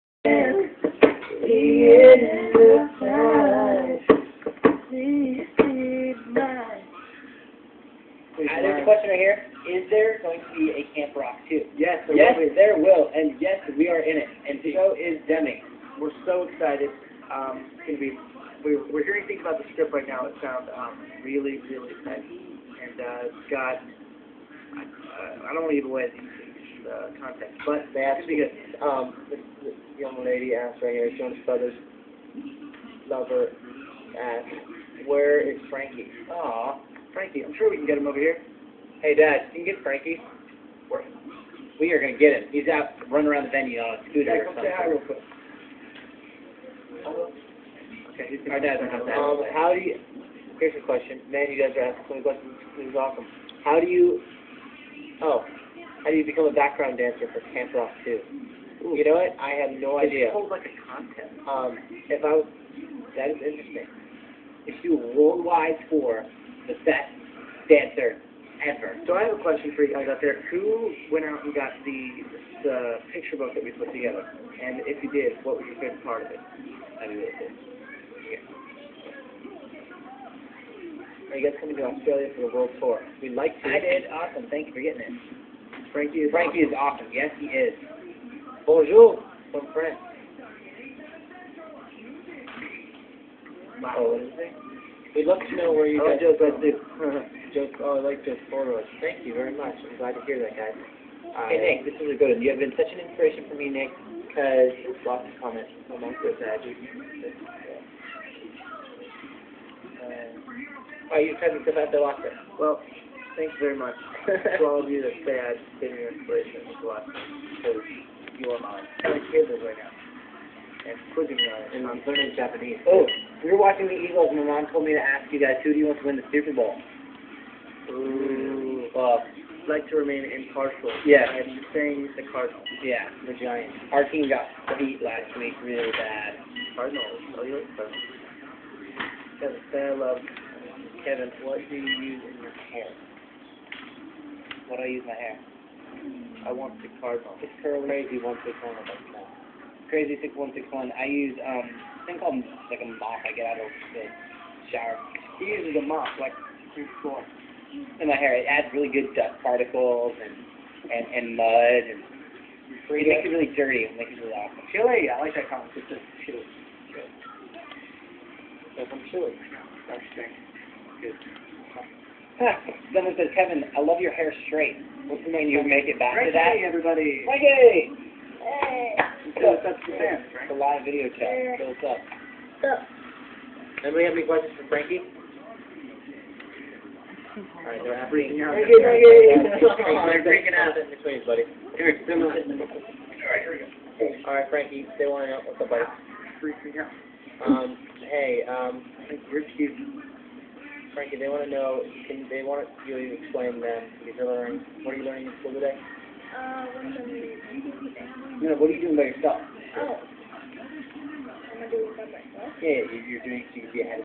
american sweetheart-Jonas brothers live chat
trying to get the computers to work.to many people trying to get in chat live with the jonas and their ones.We finally chatted with the Jonas after our videos.it was so excited. Bless you Jonas
4 videos...four of four